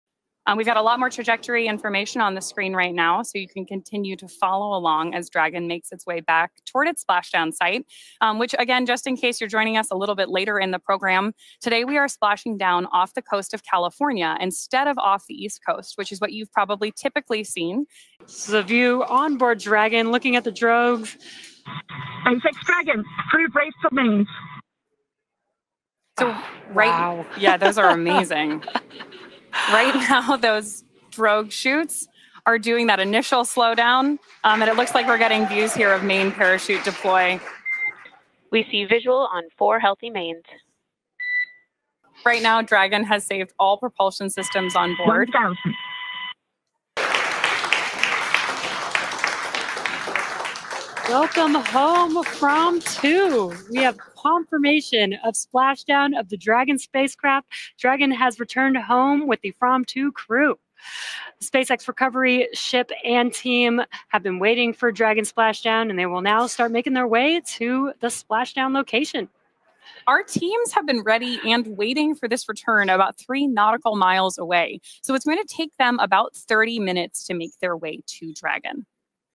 NASA Makes History with FIRST Pacific Splashdown
NASA makes history again with the first crew splashdown in the Pacific since Apollo! This extraordinary event marks a thrilling new chapter in human space exploration. After decades, a manned spacecraft has safely returned to Earth, touching down in the Pacific Ocean—a feat last witnessed during the legendary Apollo missions.

Watch never-before-seen footage of the splashdown, live crew reactions, and expert analysis on what this means for the future of space travel. From the tense moments before re-entry to the emotional celebrations aboard the recovery ship, this is the video you don’t want to miss. Whether you're a space nerd or just love epic moments in history, this one’s for you. Buckle up, because the era of space travel just got a powerful new spark.